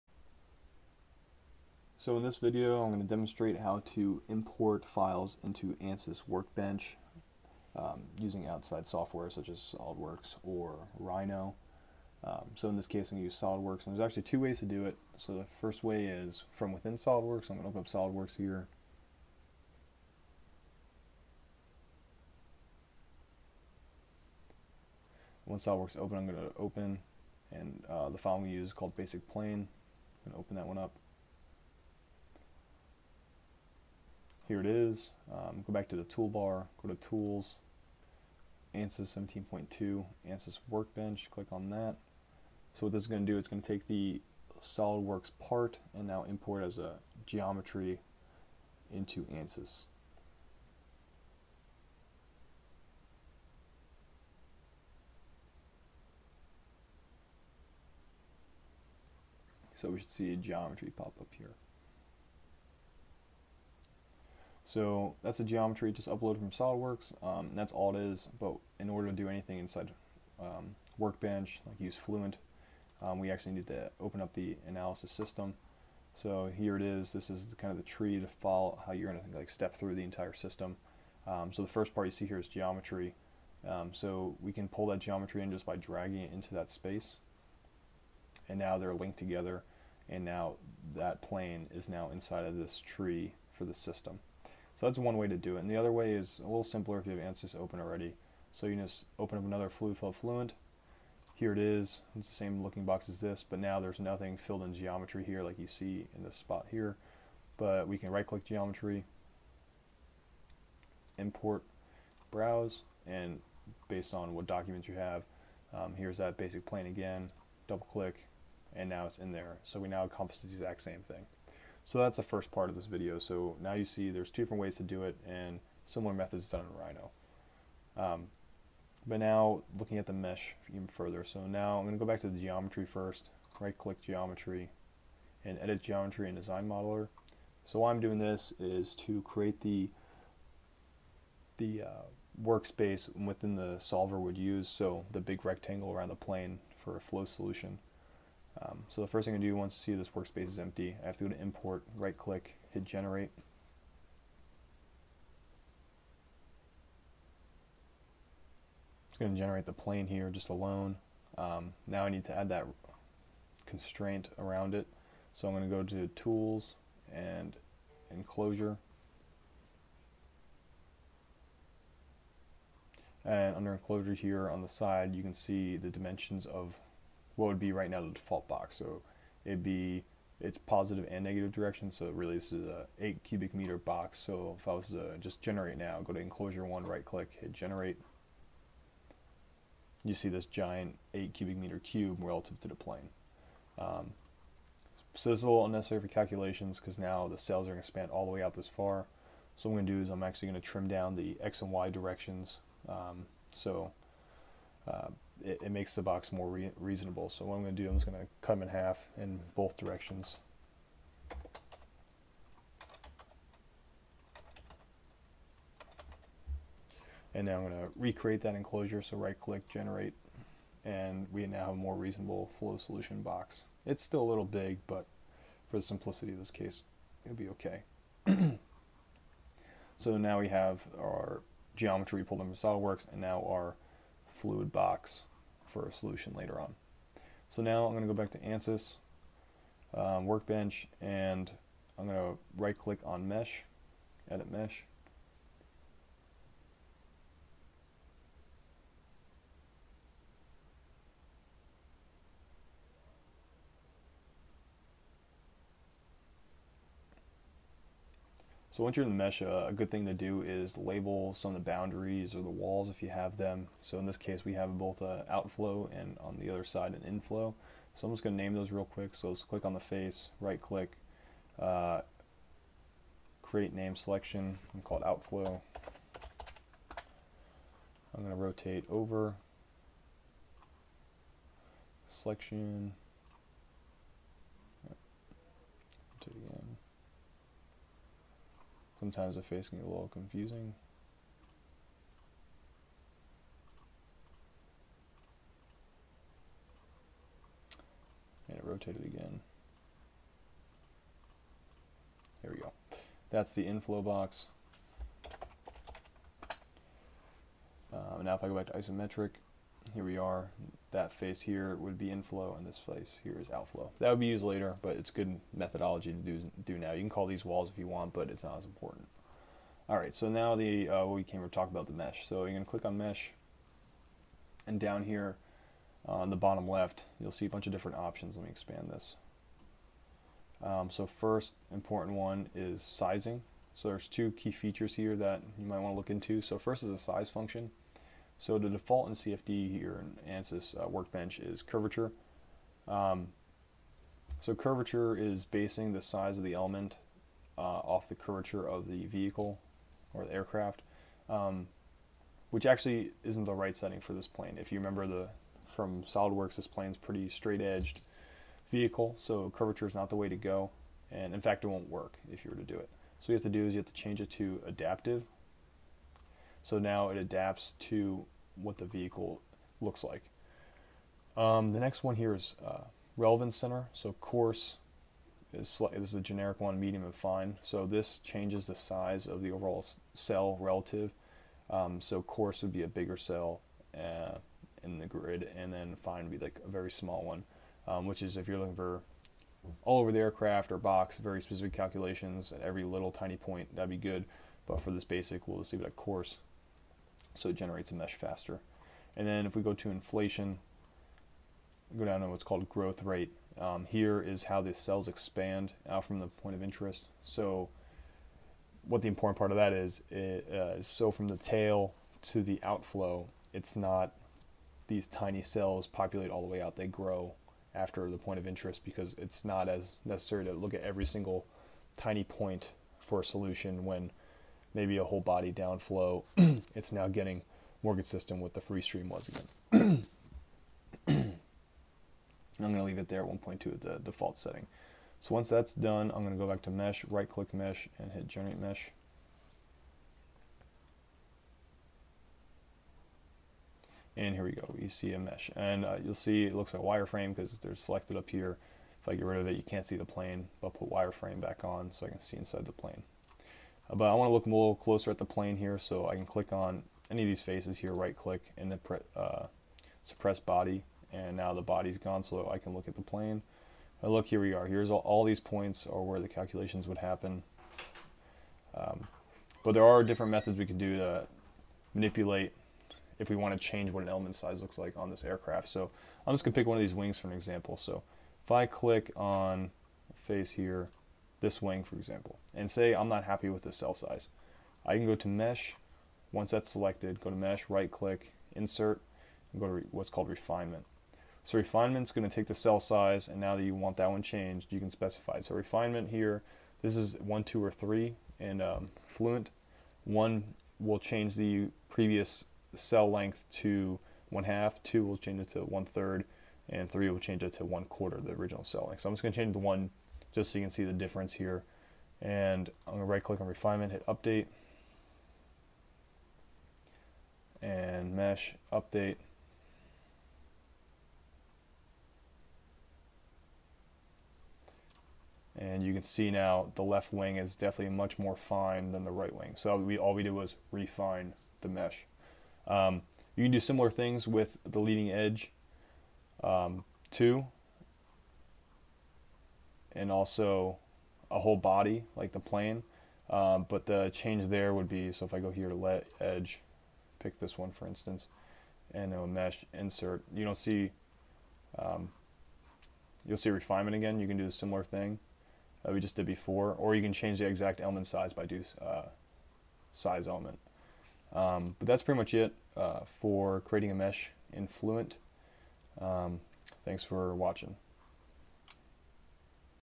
FLUENT 2.1: Import and Mesh
This video gives an introduction on how to import previously created geometries from a CAD package outside of ANSYS, such as SolidWorks or Rhino. Once the geometries were imported, the video goes through how to create an enclose and a mesh with the geometry. Various mesh settings are discussed and changed to demonstrate possible ways to fine tune the mesh.